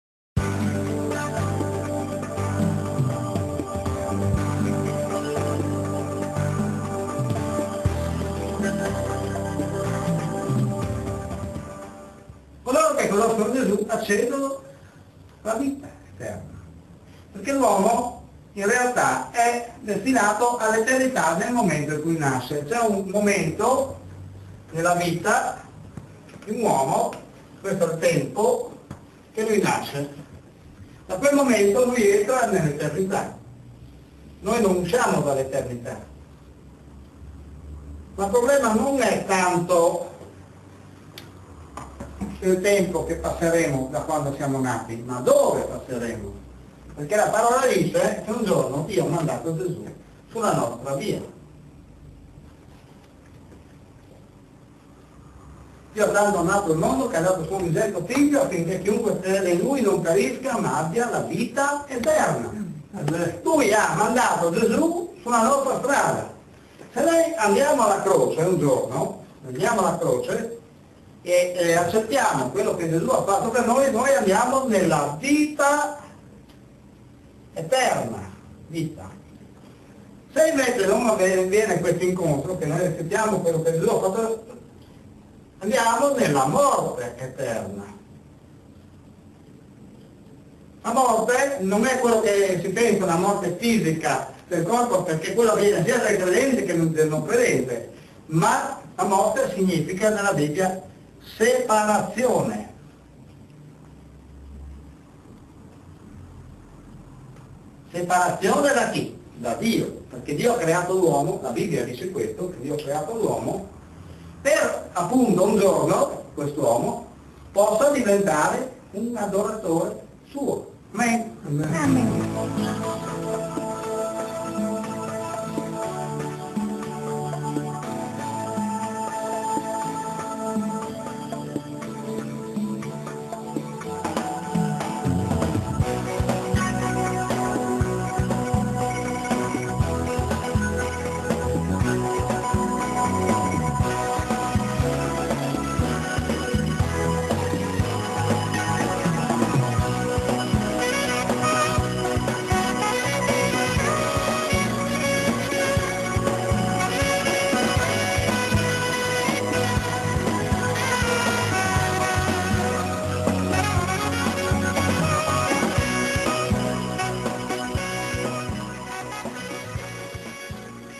Dove passerai l'eternità?
L'uomo è stato creato per diventare un adoratore di Dio.